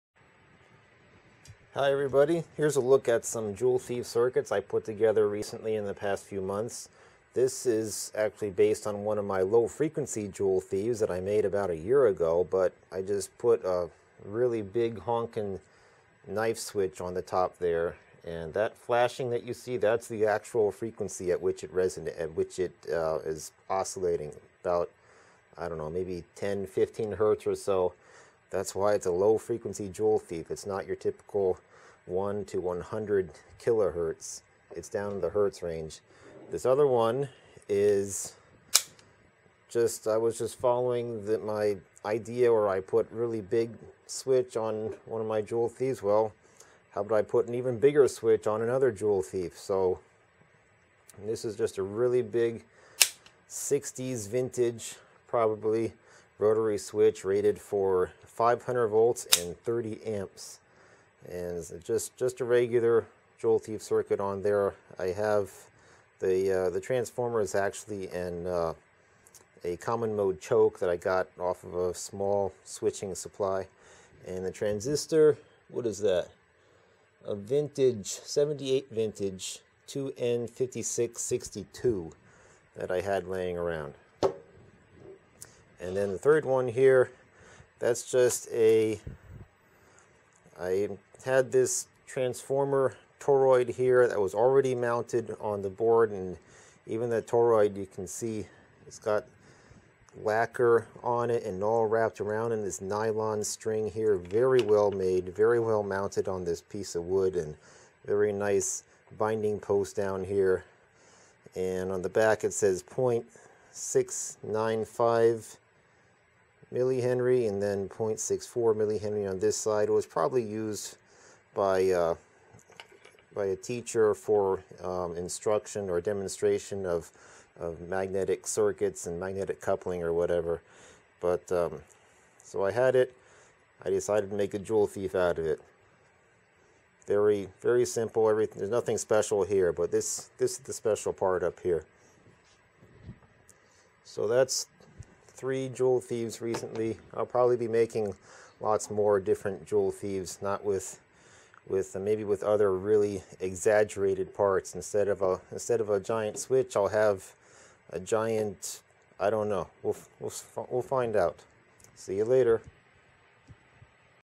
(#0095) Ordinary Joule Thief, Unusual Parts
A look at 3 different joule thieves.